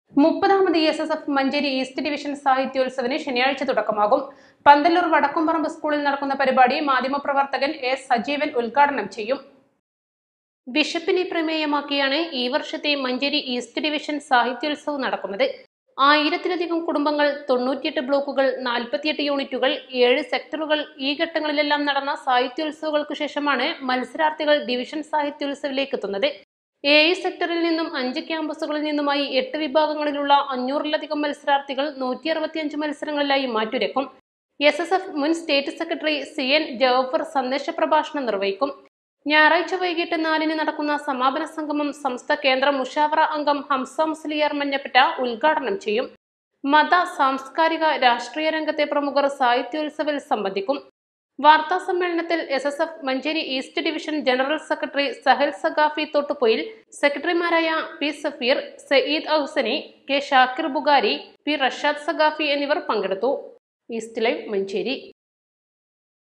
മുപ്പതാമത് എസ്.എസ്.എഫ് മഞ്ചേരി ഈസ്റ്റ് ഡിവിഷൻ സാഹിത്യാത്സവിന് ശനിയാഴ്ച തുടക്കമാകും.
The 30th SSF Manjeri East Division Literary Festival will begin on Saturday.